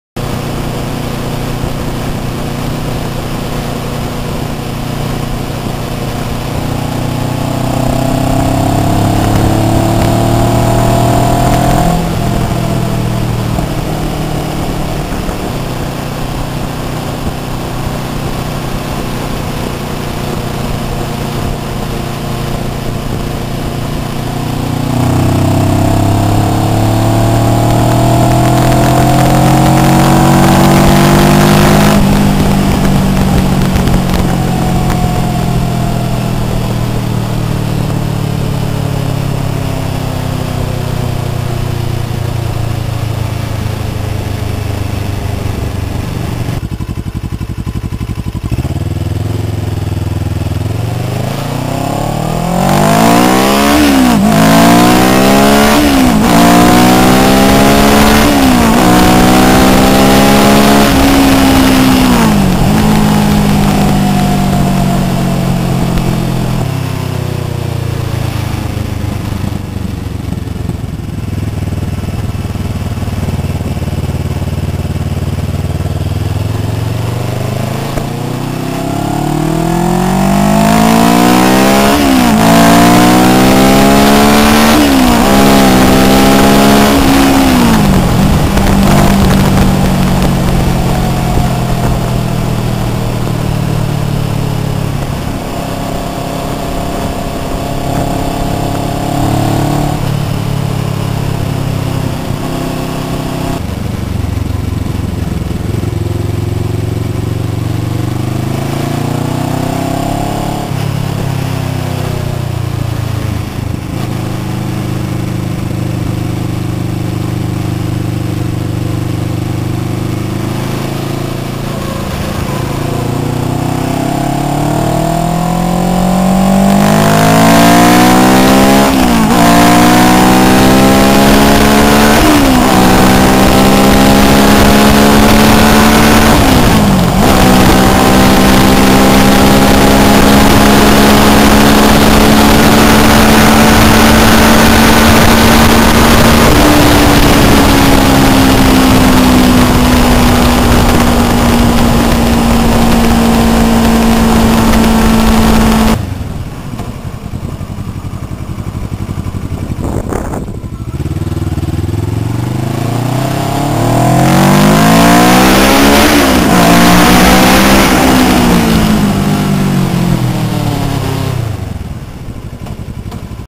ich beim motorrad fahren
hab ne kleine digicam mit kabelbinder an den lenker getüddelt xD
leider ist die qualität nicht die beste und die cam kommt manchmal auf die vibration am lenker nicht klar, aber ich denke fürs erste hat´s geklappt xD